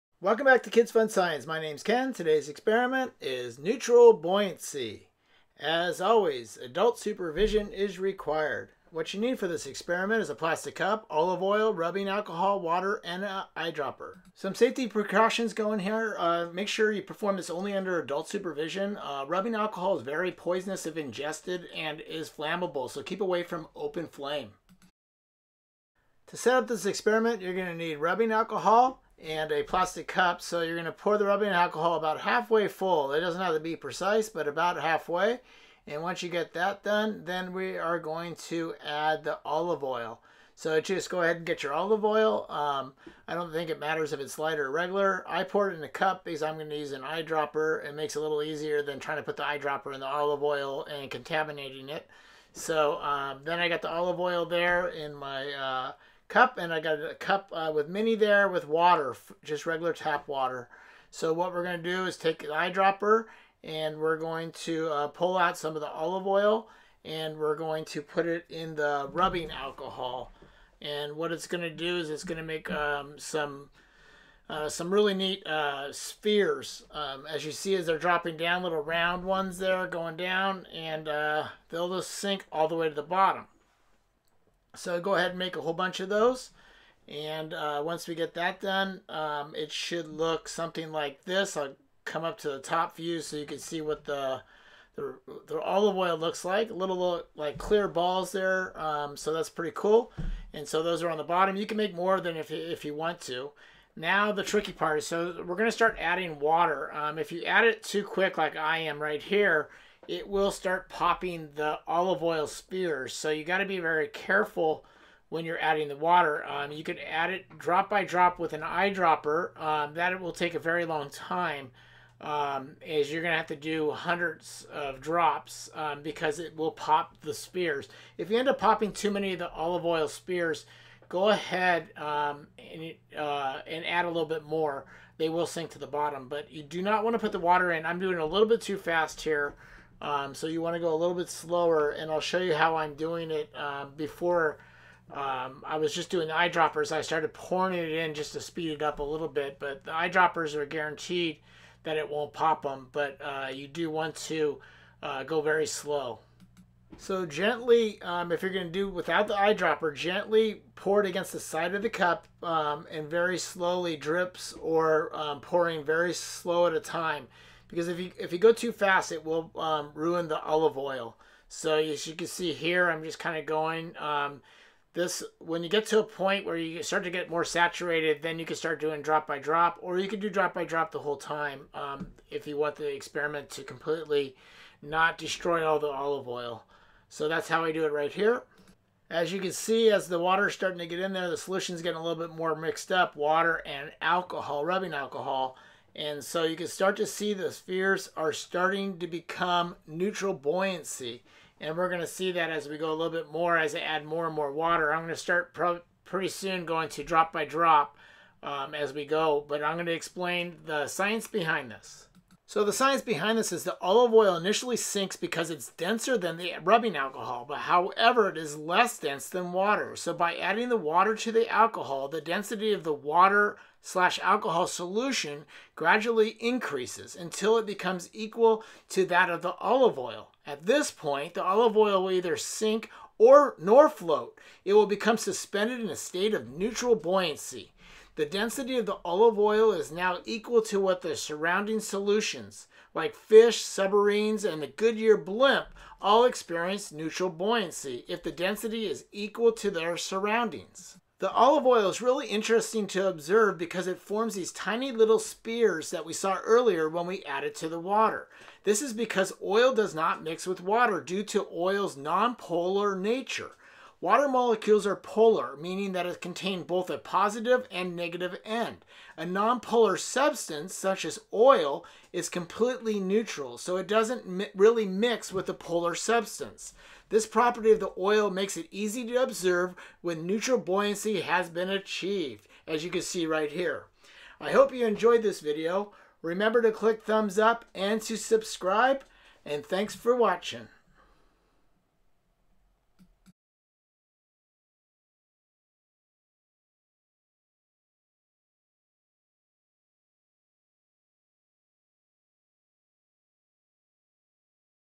How to make Olive Oil have Neutral Buoyancy experiment (STEM)
AmazonFresh Organic Extra Virgin Olive Oil, 500 ml (16.9 FL OZ)

To achieve neutral buoyancy

 Precautions: Perform only under adult supervision. Isoprophl alcohol is poisonous if ingested and is flammable. Keep away from open flame.

1.) Fill the cup about halfway with alcohol.
2.) Add several drops of olive oil to the alcohol. They will form distinct little spheres and sink to the bottom.
3.) Add water drop by drop until the balls of olive oil rise and become suspended in the middle of the water-alcohol solution.

Explanation:
The olive oil initially sinks because it is denser than the alcohol. However, it is less dense than water. By adding water to the alcohol, the density of the alcohol-water solution is gradually increased, until it becomes equal to that of the olive oil. At this point, the olive oil will neither sink nor float, but will become suspended in a state of neutral buoyancy. The density of the olive oil is now equal to that of the surrounding solution. Fish, submarines, and the Goodyear blimp can all experience neutral Buoyancy if their density is equal to that of their surroundings.

The olive oil is interesting to observe because it forms many tiny little spheres when it it added to the water. The oil does not mix with water due to oil’s non polar nature. Water molecules are polar, meaning they contain both a positive and negative end. A non-polar substance, such as oil, is completely neutral, so it does not readily mix with a polar substance. This property of the oil makes it easy to observe wet neutral buoyancy has been achieved.

Neutral Buoyancy experiments

Neutral Buoyancy experiments chemistry